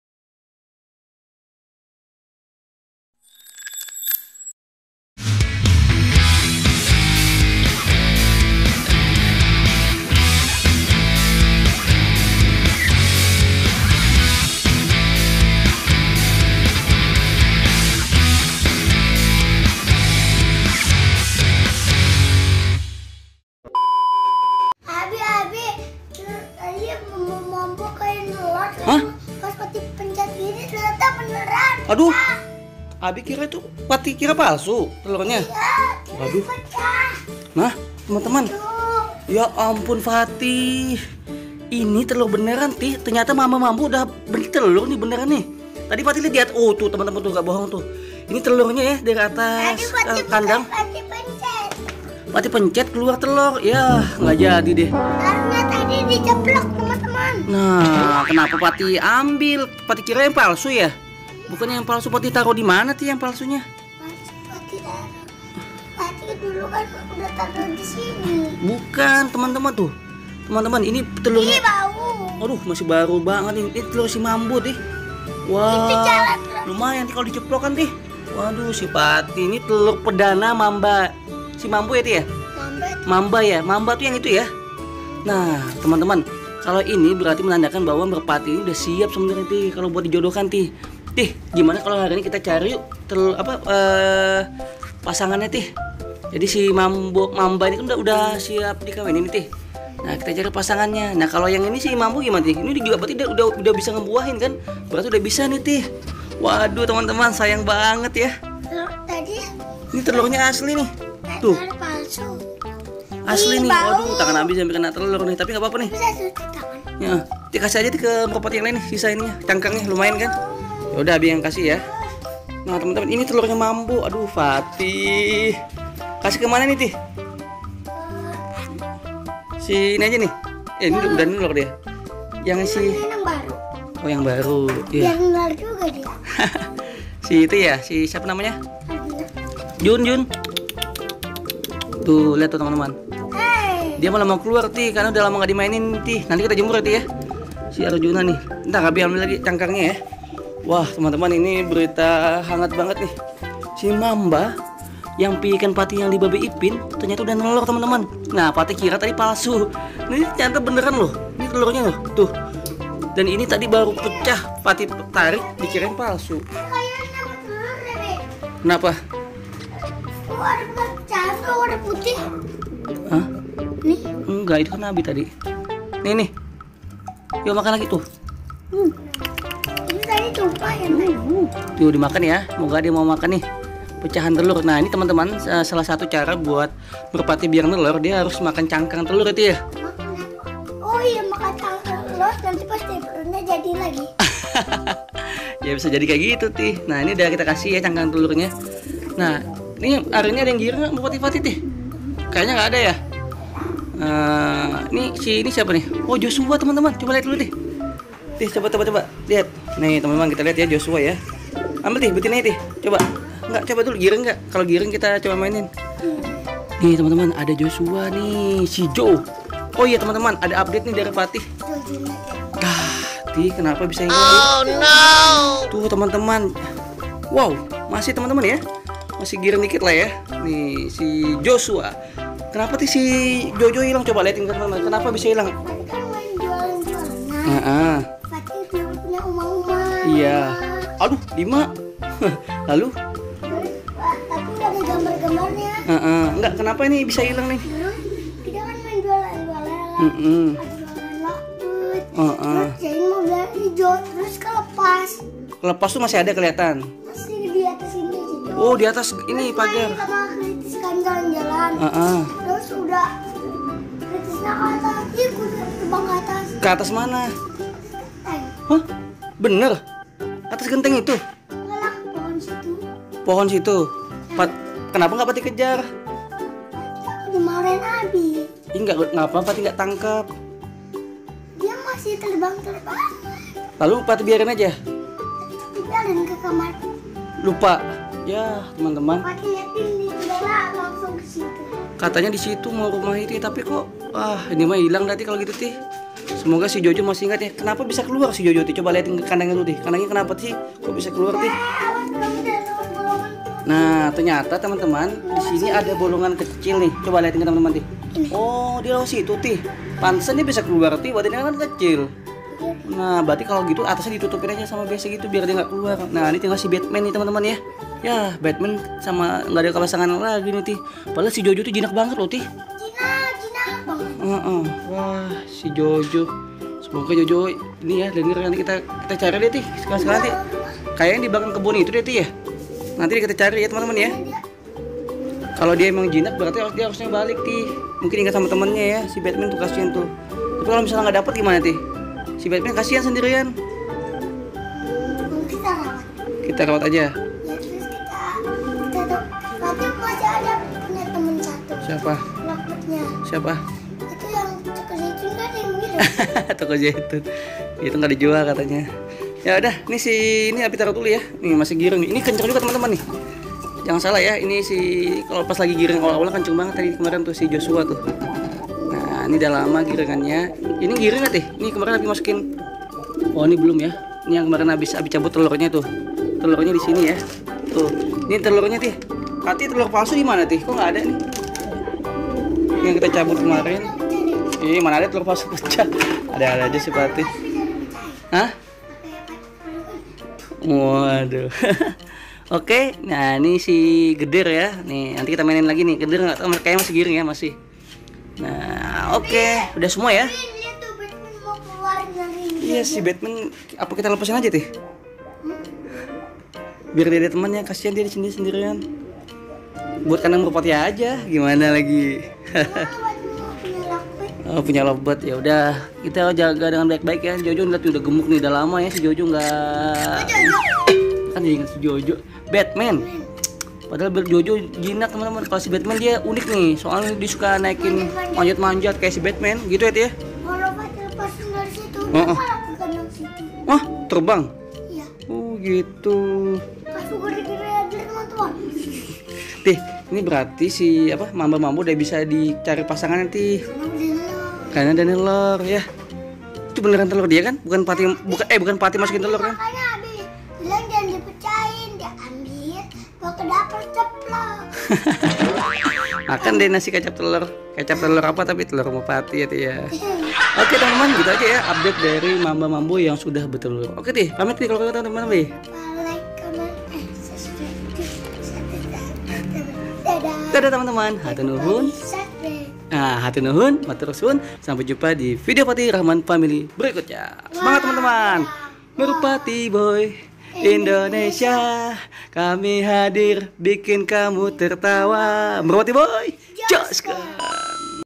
Perdana Akhirnya Merpatiku Mamba Mambo BERTELUR | Dipecahkan Fatih??
Assalamualaikum

halo teman teman ada info mepati Fatih si mamba mambo udah bertelur satu. tapi Karena Fatih Kita itu teur palsu akhirnya pecah. gimana videonya simak ya .